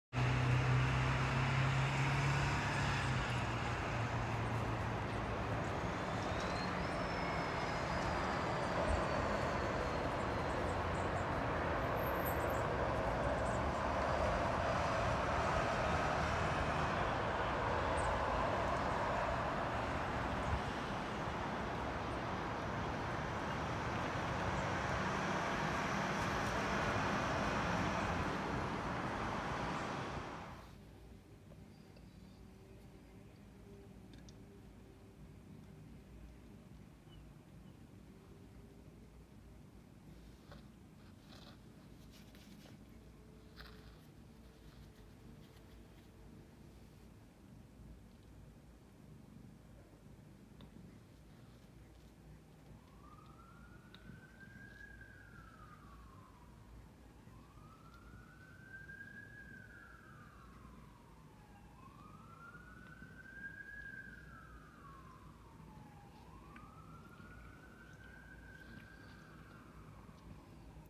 The Rut, fighting over the ladies, Poor Souls
Two Bucks fight over a Doe in Victoria BC